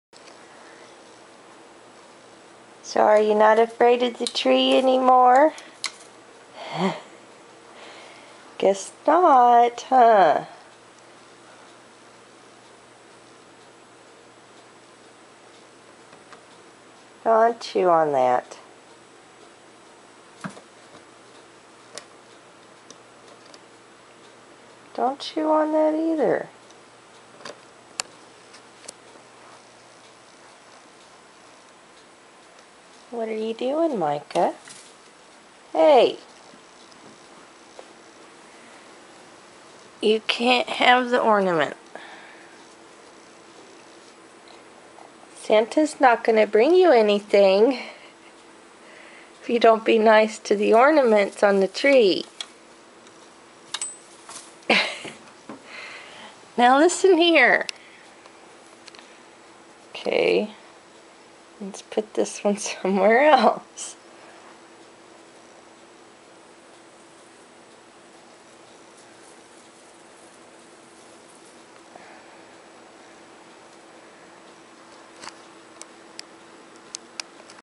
Micah keeps getting ornament
He kept getting the snowflake ornament off so I had to move it.